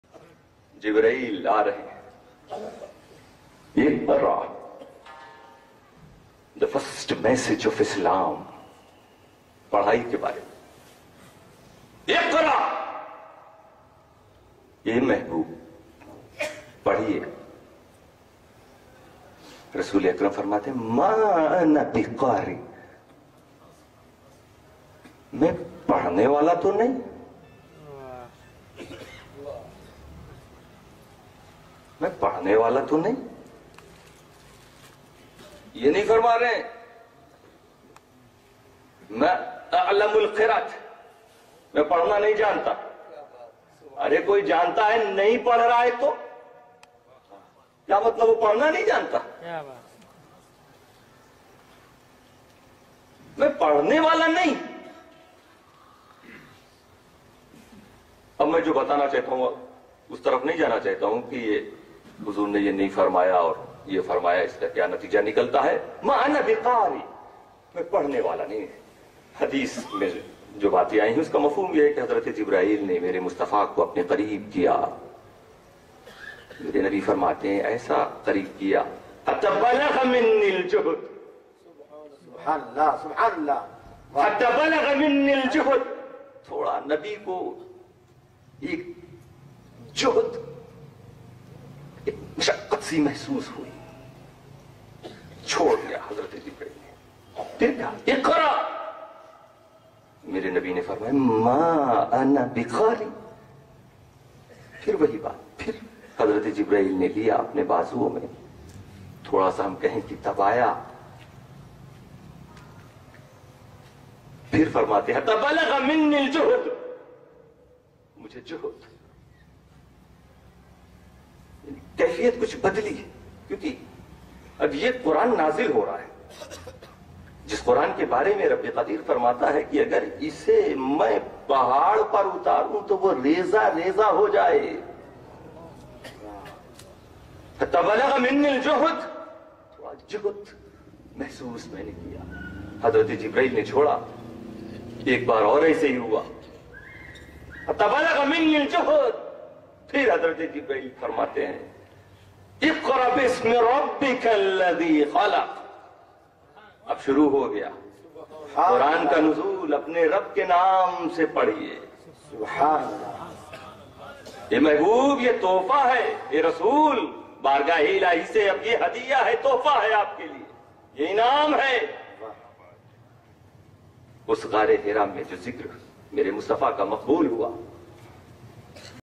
QURAN SHARIF KA NUZUL KESE HUVA || HUZOOR FAZILE BAGHDAD SYED HASAN ASKARI ASHRAF
TOPIC :- QURAN SHARIF KE NUZUL KA KUKHTASAR WAQIYA

MUQARRIR :- HUZOOR FAZILE BAGHDAD ALLAMA SYED HASAN ASKARI ASHRAF IBN ARBI ASHRAF ASHRAFIYUL JEELANI KICHOCHVI

JAROOR SUNE OR SHARE KARE...